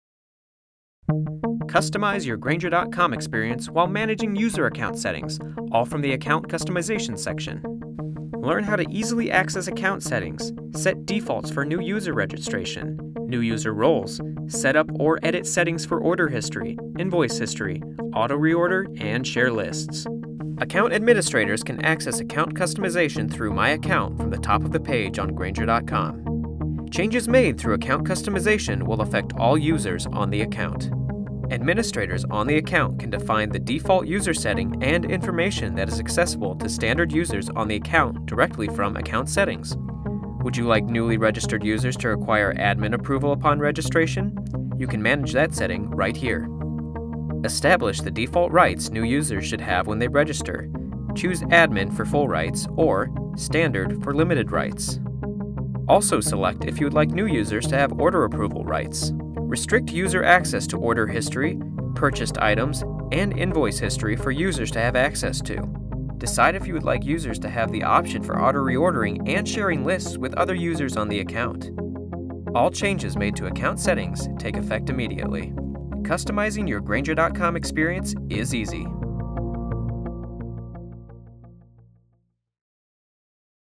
Account Settings
Customize your Grainger.com® experience while managing user account settings all from Account Customization section.  Learn how to easily access Account Settings,  set defaults for new user registration, new user roles, set up or edit settings for order history, invoice history, auto-reorder and share Lists.